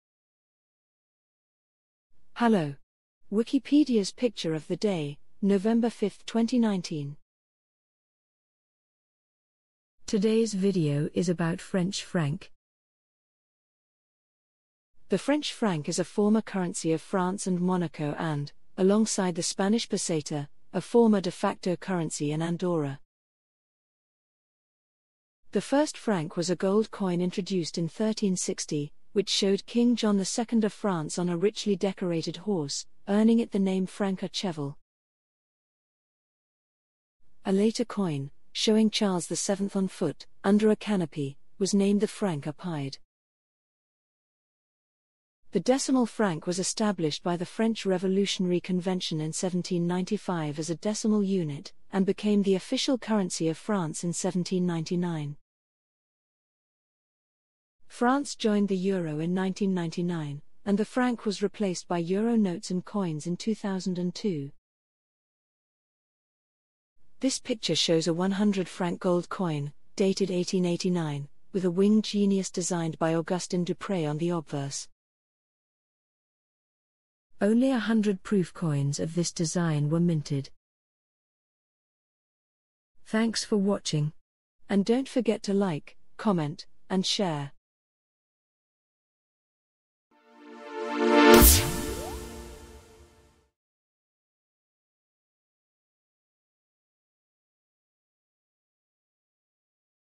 Wikipedia Picture of the Day: 2019-11-05 - French franc (Narrated by Amy)
Wikipedia Picture of the Day - November 5th, 2019 - French franc (Narrated by Amy)

The French franc is a former currency of France and Monaco and, alongside the Spanish peseta, a former de facto currency in Andorra. The first franc was a gold coin introduced in 1360, which showed King John II of France on a richly decorated horse, earning it the name franc à cheval. A later coin, showing Charles VII on foot, under a canopy, was named the franc à pied. The decimal franc was established by the French Revolutionary Convention in 1795 as a decimal unit, and became the official currency of France in 1799. France joined the euro in 1999, and the franc was replaced by euro notes and coins in 2002.
This picture shows a 100-franc gold coin, dated 1889, with a "winged genius" designed by Augustin Dupré on the obverse. Only a hundred proof coins of this design were minted.

Coin credit: Monnaie de Paris; photographed by the National Numismatic Collection